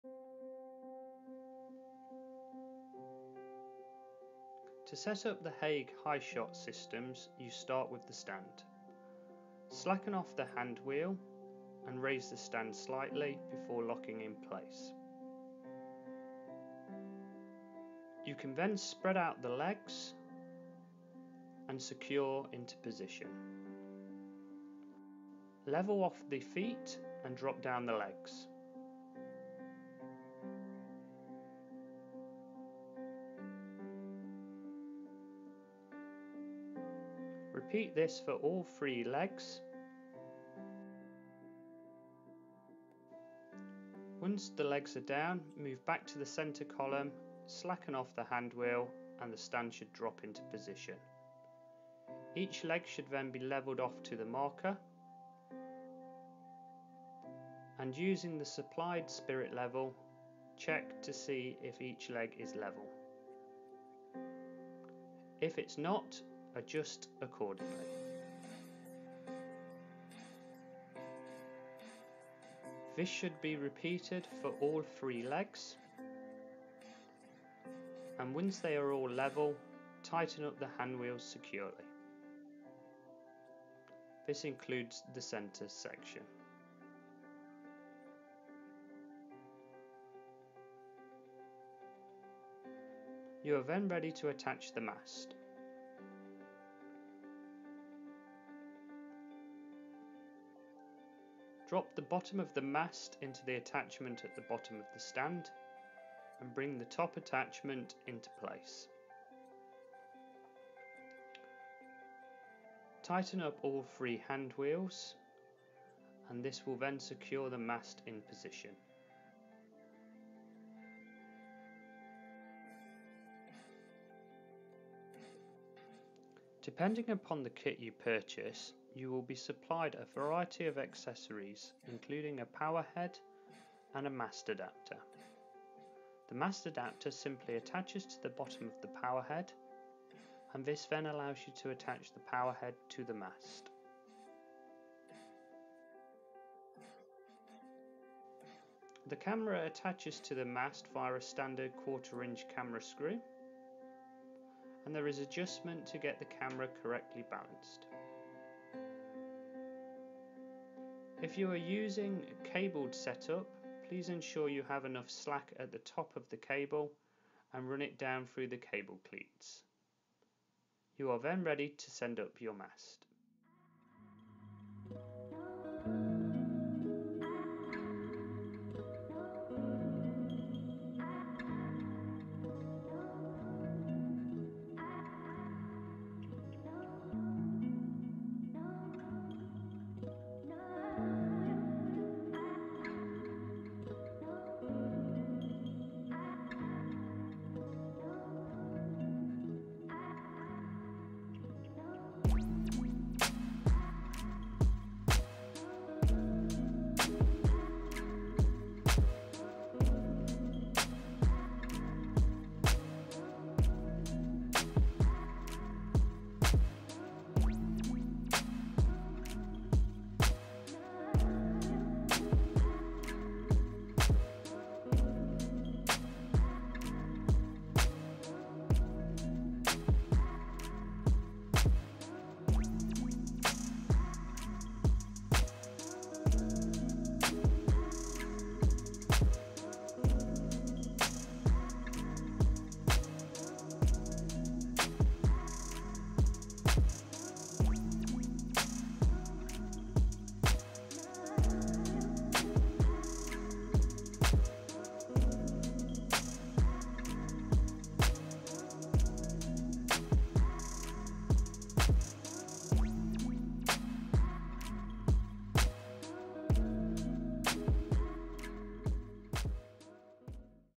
Hague Highshot Camera Mast For Filming, Photography & Sports Analysis
The Hague Highshot Aerial Camera Mast is ideal for filming from a height, aerial photography and Sports Analysis.

Available in a variety of heights the Hague Highshot systems include remote controlled powerheads, wireless options and can be tailored to your individual requirements.

Get above your competition with the Hague Highshot Elevated Camera Mast.

Hague Camera Supports | Nottingham | England